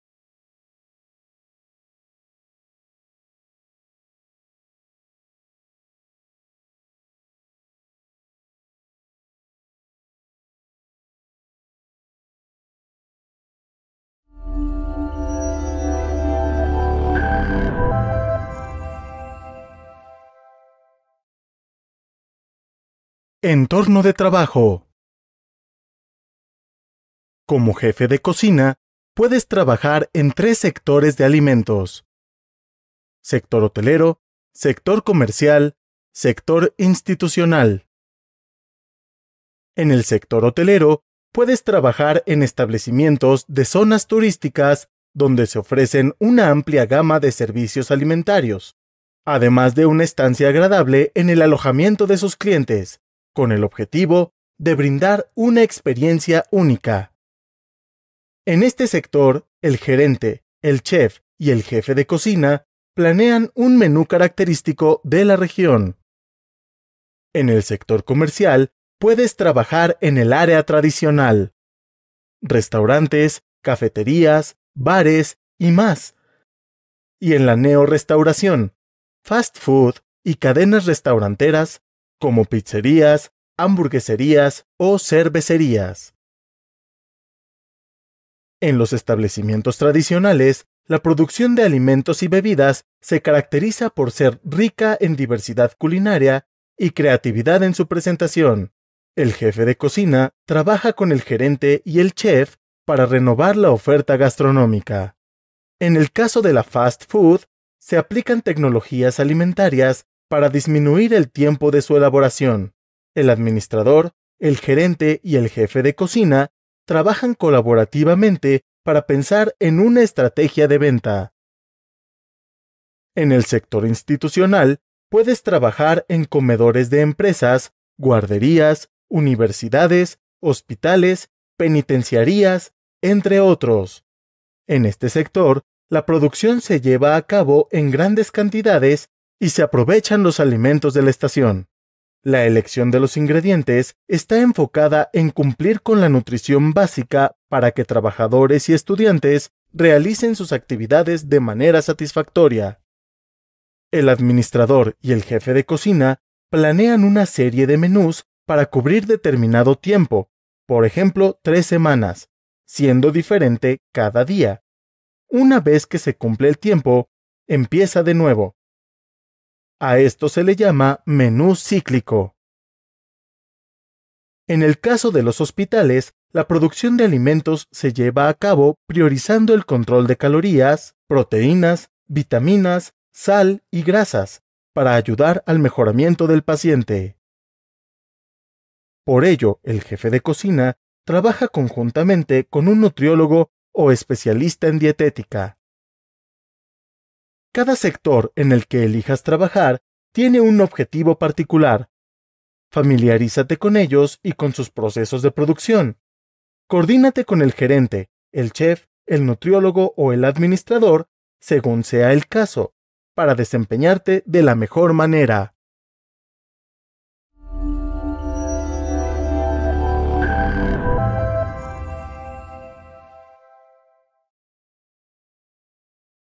Curso jefe de Cocina / Capitulo 01: Entorno de trabajo
El Jefe de cocina en un restaurante o cualquier otra entidad donde se ofrezcan servicios de alimentos y bebidas tiene la responsabilidad de planificar, organizar, dirigir y controlar todos los procesos que ocurren desde el momento en que se realiza la mise en place hasta el cierre de las operaciones diarias, asi como supervisar y proteger a cada uno de sus empleados. En este curso te compartimos todo lo relacionado con el papel del jefe de cocina.